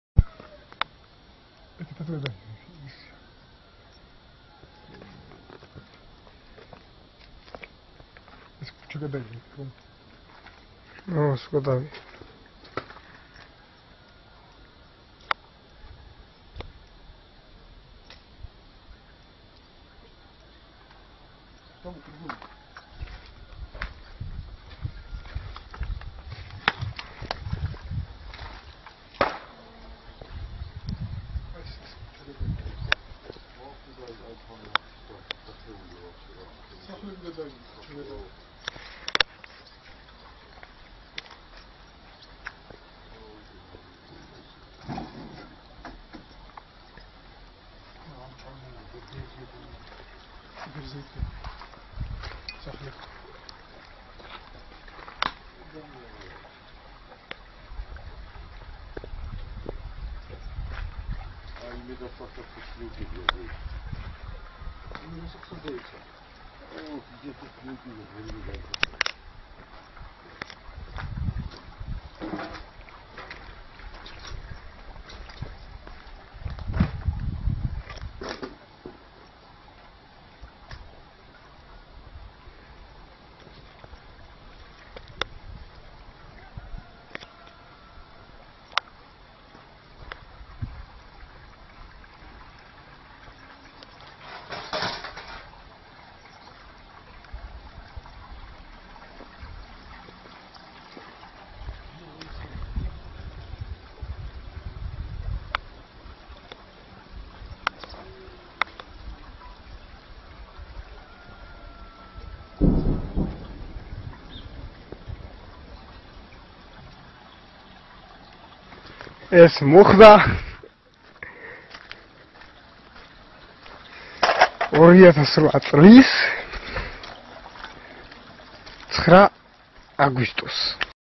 Fierce fighting around the town of Shotlanli in Nagorno-Karabakh
Fierce fighting around the town of Shotlanli in Nagorno-Karabakh between the armies of Armenia and Azerbaijan leaves between 20 and 30 civilians dead and an unknown number of injured.